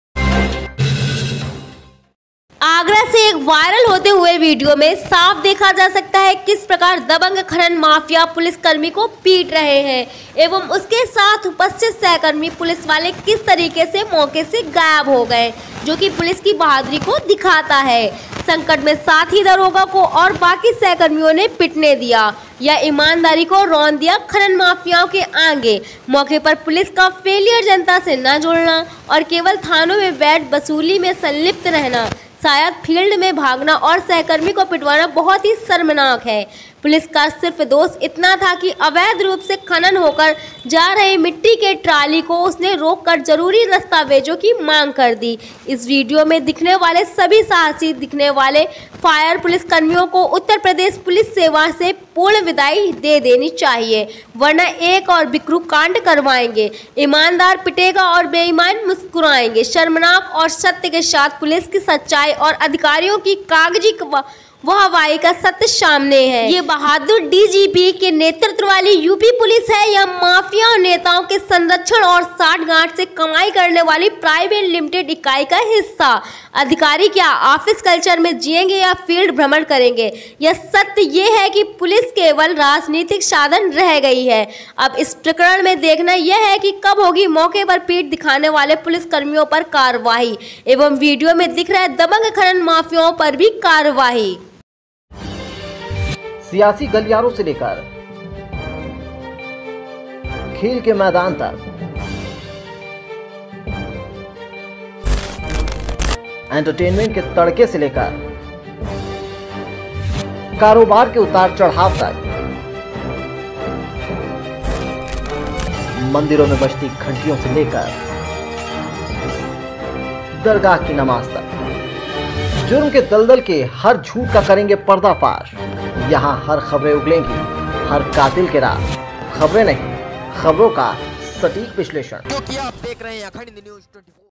आगरा में एक बार फिर दिखी पुलिस की लापरवाही खनन माफिया पिटते रहे पुलिस को सहकर्मी जान बचा कर भागे
आगरा से एक वायरल होते वीडियो में साफ देखा जा सकता है किस प्रकार दबंग खनन माफिया पुलिसकर्मी को पीट रहे एवं उसके साथ उपस्थित सहकर्मी पुलिस वाले किस तरीके मौके से गायब हो गए जो कि पुलिस की बहादुरी को दिखाता है

संकट में साथी को दारोग़ा और बाक़ी सहकर्मियों ने पिटने दिया या ईमानदारी को रौंद दिया खनन मफ़ियाओ के आगे?मौक़े पर पुलिस का फेलियर जनता से न जुड़ना और केवल थानो मे बैठ वसूली में संलिप्त रहना है शायद!फ़ील्ड में भागना और सहकर्मी को पिटवाना बहुत ही शर्मनाक है

पुलिसकर्मी का सिर्फ दोष इतना था कि अवैध रूप से खनन होकर जा रही मिट्टी के ट्राली को उसने रोक कर जरूरी दस्तावेजों की मांग कर दी

इस विडीओ में दिखने वाले सभी साहसी दिखने वाले कायर पुलिसकर्मियो को उत्तरप्रदेश पुलिस सेवा से पूर्ण विदाई दे देनी चाहिए वर्ना एक और बिकरु कांड करवाएँगे ! ईमानदार पिटेगा और बेईमान मुस्कुराएँगे! शर्मनाक और सत्य के साथ पुलिस की सच्चाई और अधिकारियों की कागजी वाहवाही का सत्य सामने है !

ये एक बहादुर dgp के नेतृत्व वाली Uppolice है या मफ़ियाओ ,नेताओ के संरक्षण और साँठगाँठ से कमाई करनेवाली प्राइवेट लिमिटेड इकाई का हिस्सा ? अधिकारी क्या ऑफ़िस कल्चर में जियेंगे या फ़ील्ड भ्रमण करेंगे ? या सत्य ये है की पोलिस केवल राजनीतिक साधन रह गयी ।

अब इस प्रकरण में देखना यह है कब होगी मौके पर पीठ दिखाने वाले पुलिसकर्मियों पर कार्यवाही एवं वीडियो में दिख रहे दबंग खनन माफियाओं पर भी कार्यवाही  ?